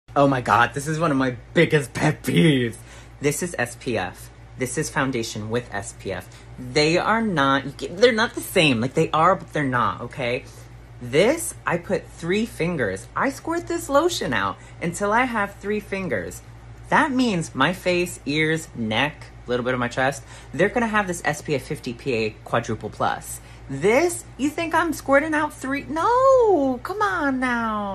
Spf from Makeup is enough
Spf from foundation is enough? Unless you are squirting out 3 fingers of your foundation and rubbing it all over your face eyes ears neck and chest the answer is NO. Think of spf as a shield you rub on, with foundation we never use that much because not only is it uncomfortable more than likely it will appear cakey and rub all over the place. Also when you go to reapply your spf (every 2 hours) you can do so easily with your typical sunscreen. Are you trying to tell me you would honestly do your whole makeup routine to get spf protection again?! The answer is no. If the answer is yes god bless you, whipping out primer foundation contour bronzer powder blush setting spray every 2 hours. Lord child you have dedication!!!